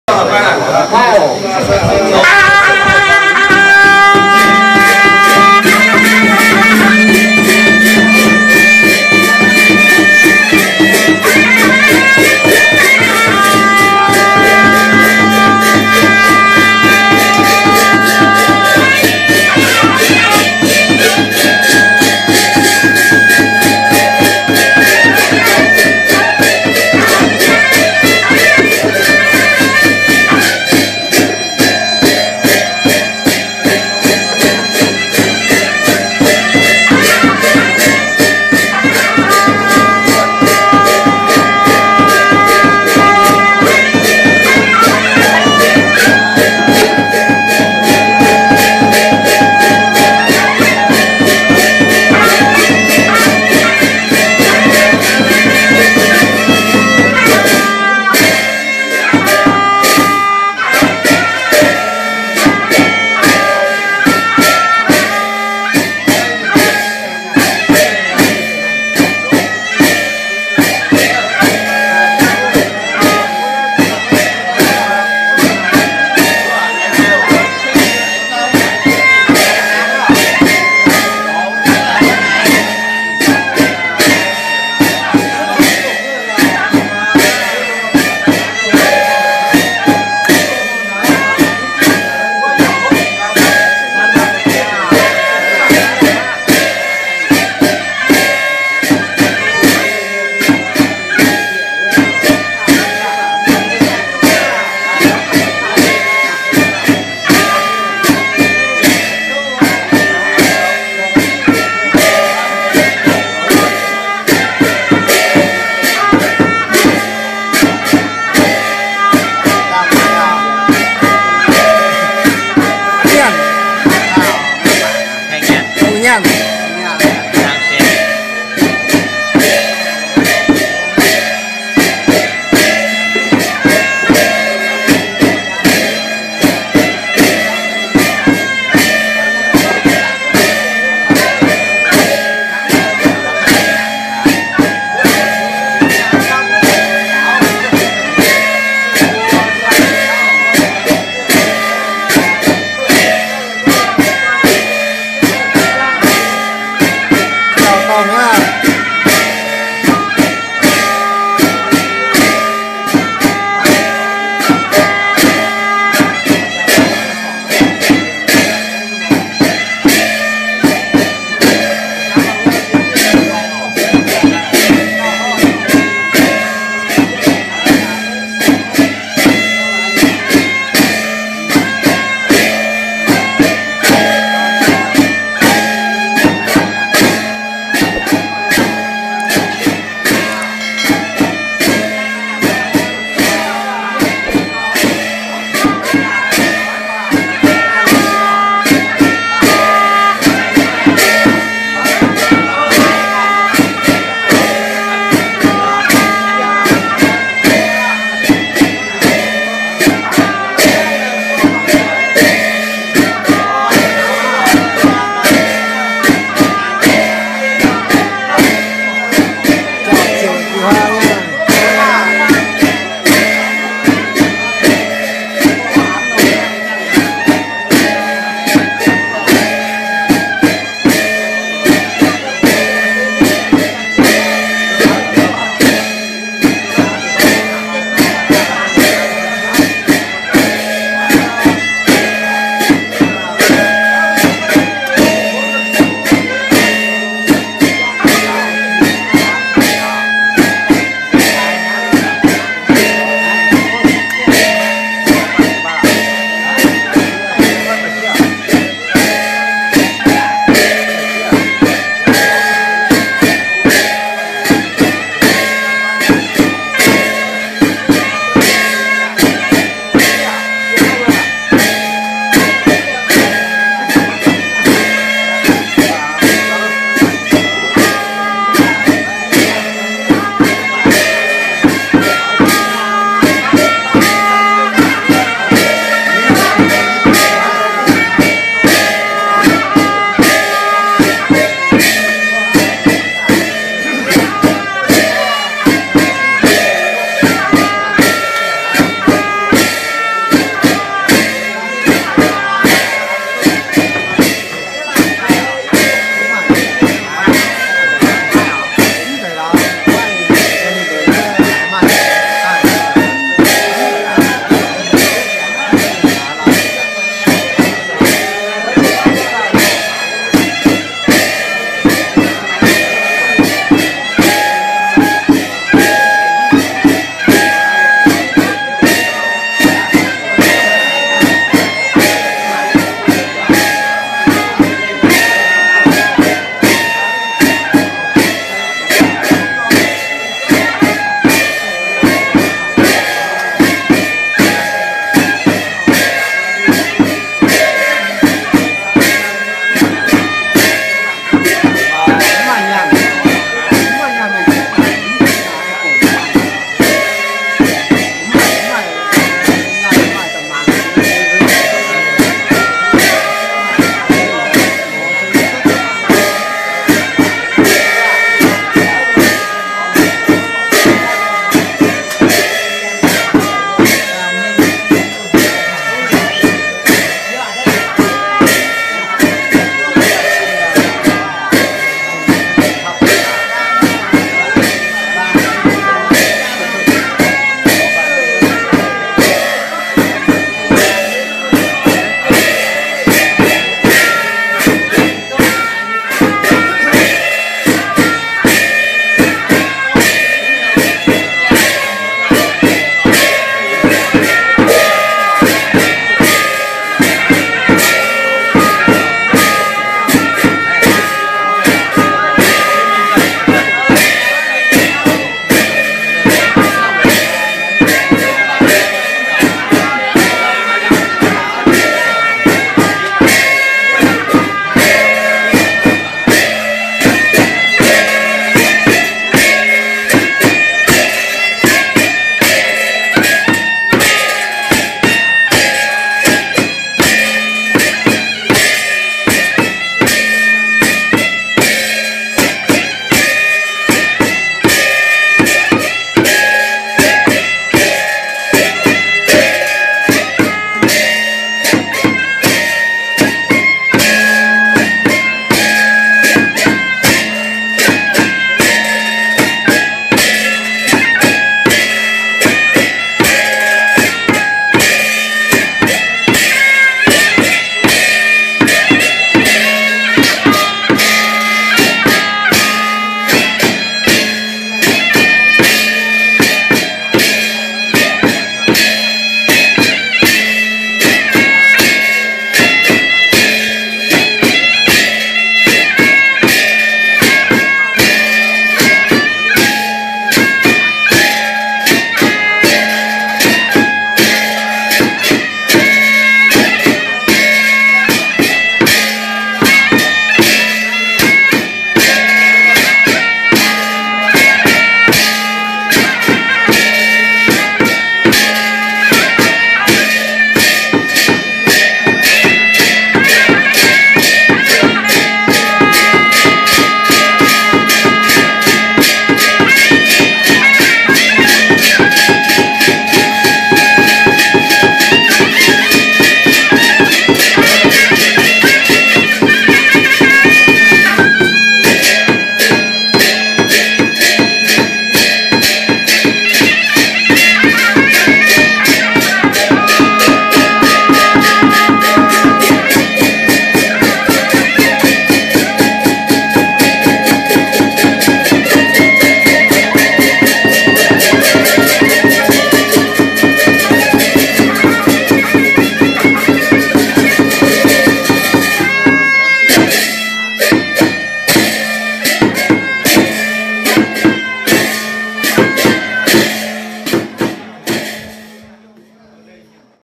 Thổi kèn đánh trỗng dân tộc dao đỏ hay nhất từ trước đến nay
Thổi kèn đánh trỗng dân tộc dao đỏ hay nhất
Bản sắc văn hóa dân tộc dao đỏ